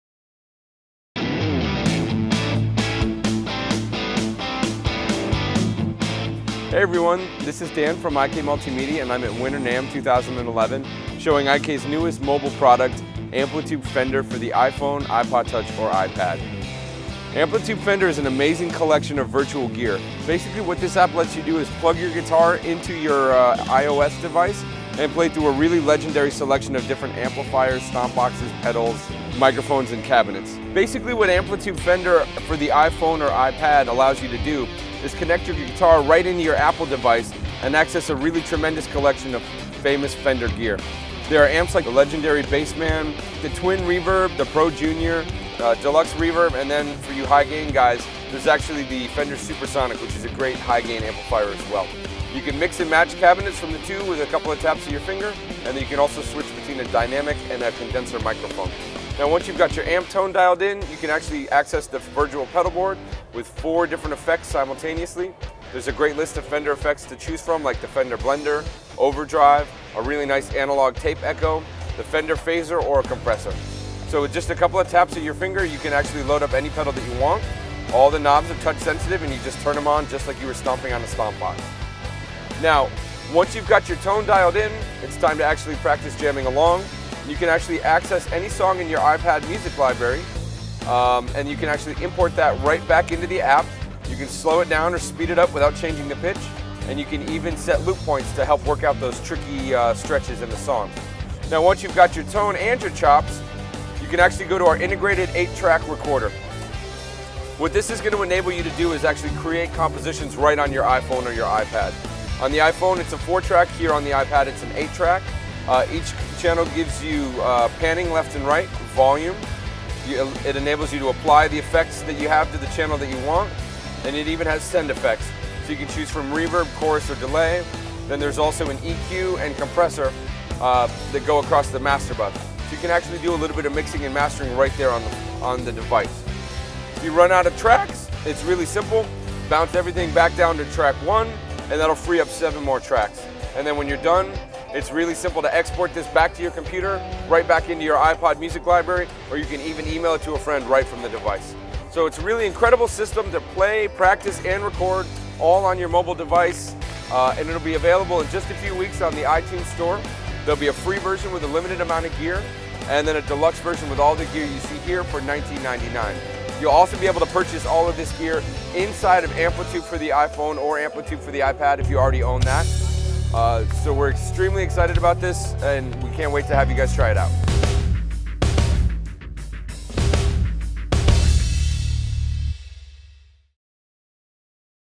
Featuring Fender Collection for iPhone/iPad and iRig at Winter NAMM 2011
IK Multimedia and Fender presents Fender Collection. Get that legendary Fender tone on your iPhone, iPod Touch or iPad.

Making it's first debut at Winter NAMM Show, IK Multimedia's own Dan Boatman shows you the in's and out's of Fender Collection for your iOS device.

Fender Collection was developed with careful guidance and critical tone testing from the tone gurus at Fender to ensure the authenticity of the sound. The app offers 5 Fender amp models — the '65 Deluxe Reverb™, the Super-Sonic™, the '65 Twin Reverb™, the '59 Bassman™ LTD and the Pro Junior™ , and 6 Fender® stompbox models — Tape Delay, Compressor, the Blender™, Phaser, Overdrive and Noise Filter.